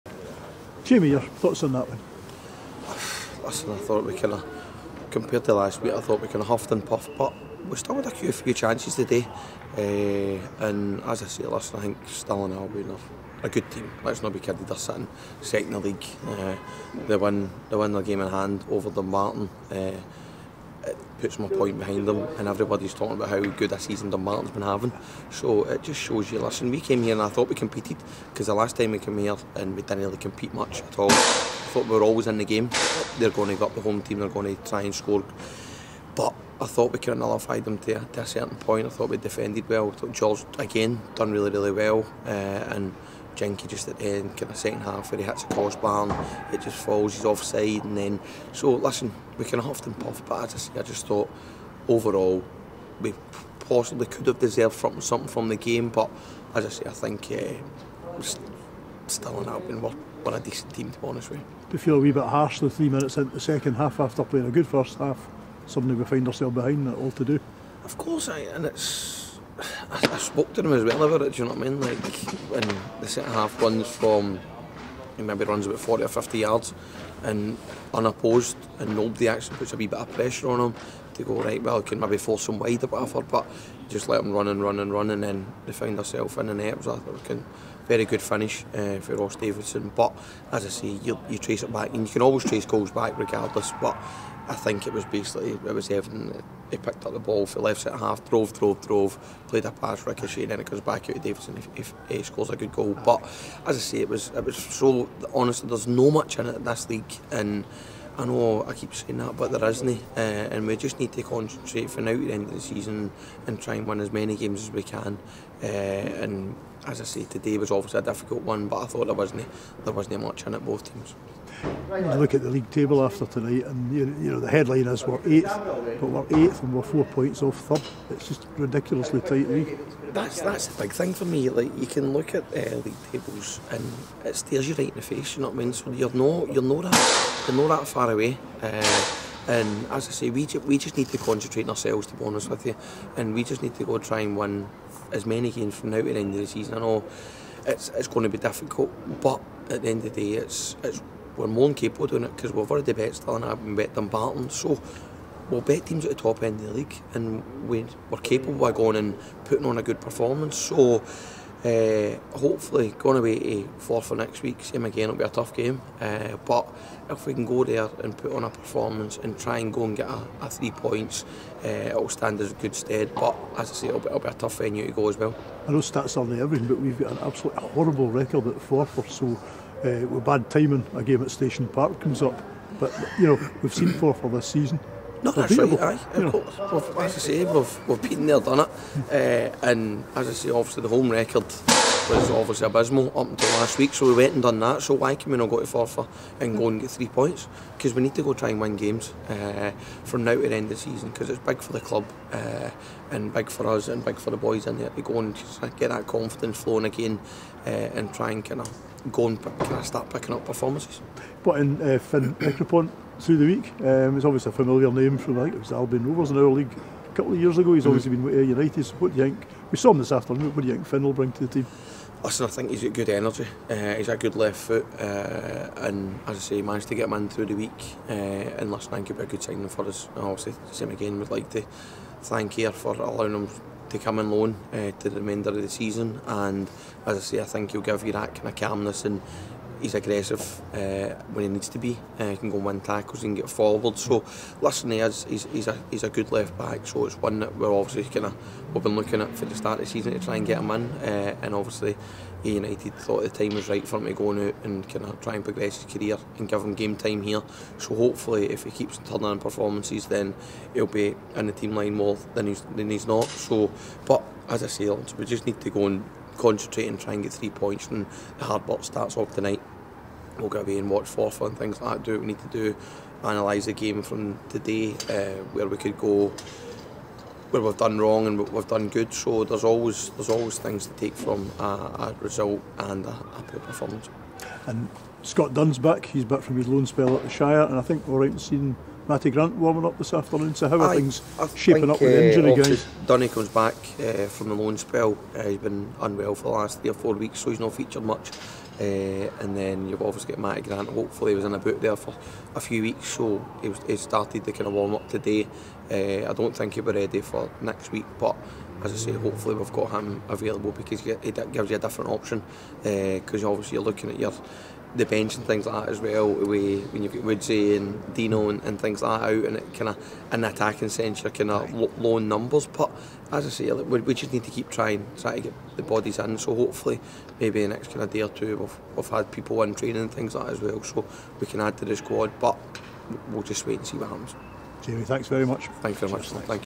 Interviews | Stirling Albion 1-0 Stranraer, 28th January 2023
Jamie Hamill speaks to Laurence Nelson following the blues 1-0 defeat away to Stirling Albion.